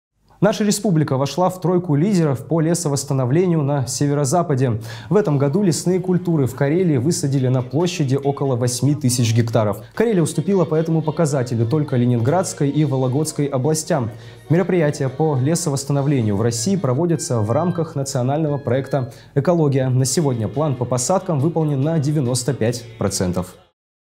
Карелия вошла в тройку лидеров по лесовосстановлению на Северо-Западе
Республика уступила по этому показателю только Ленинградской и Вологодской областям.